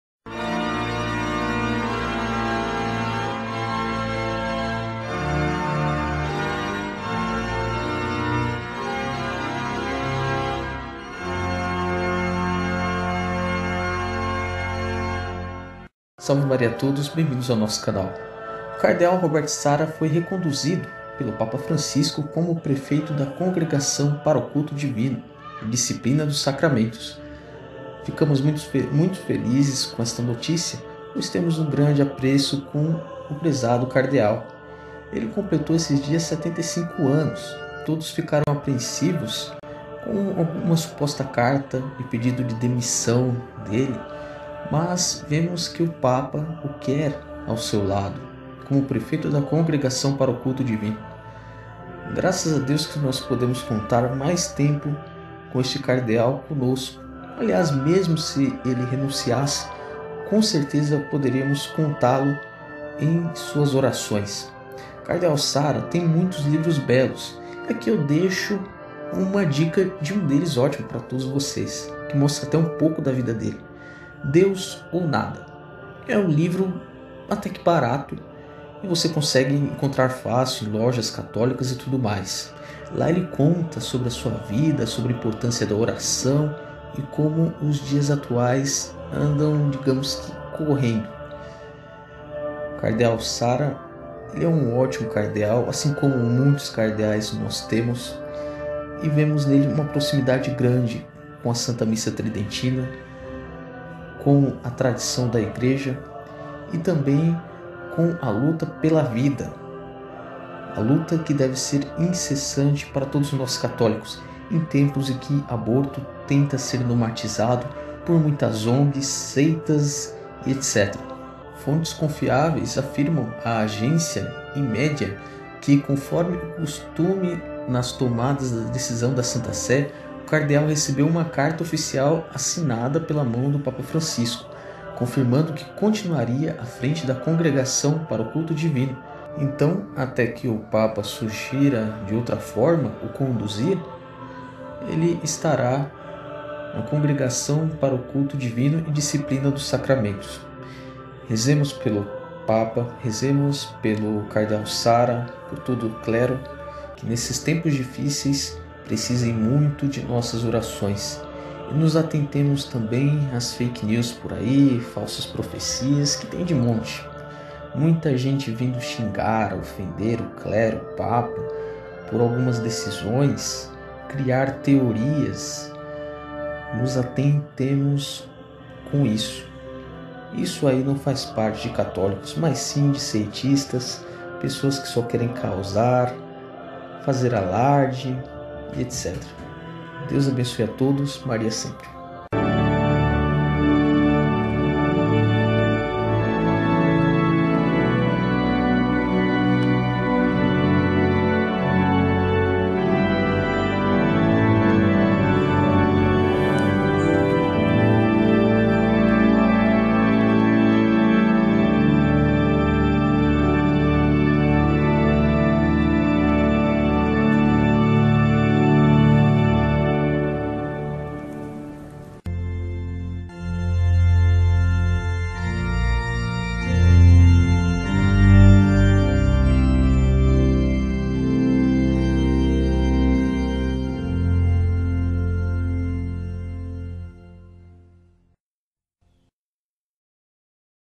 Cardeal Sarah não renunciará! Permanece como Prefeito da Congregação para o Culto Divino.
O Cardeal continua sua missão. Rezemos por ele e por todo o clero.